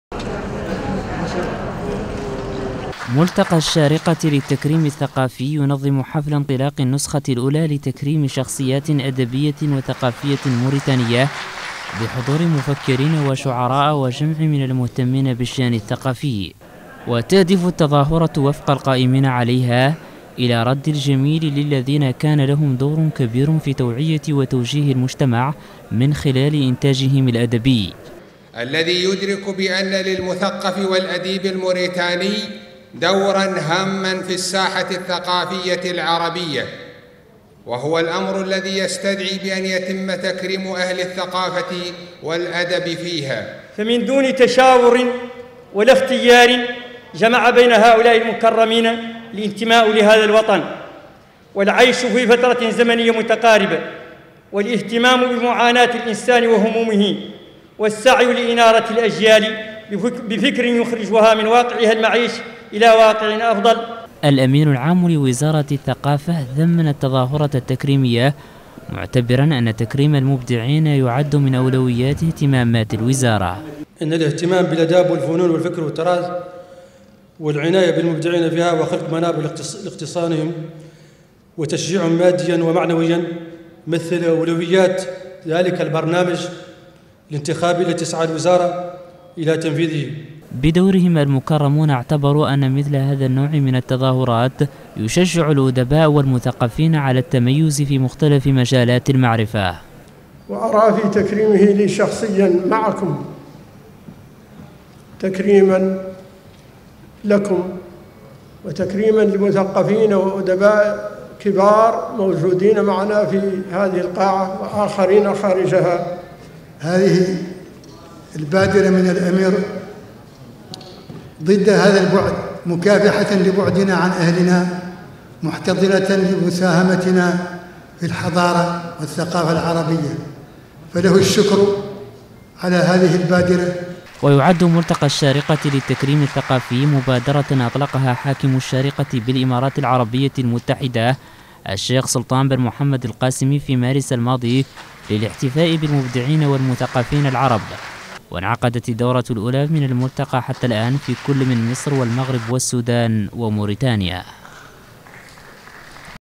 ملتقى الشارقة يكرم شعراء ومثقفين في موريتانيا
تقرير : عبد الله الحاج سيدي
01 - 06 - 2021
قناة المرابطون - الصفحة الرسمية - 2020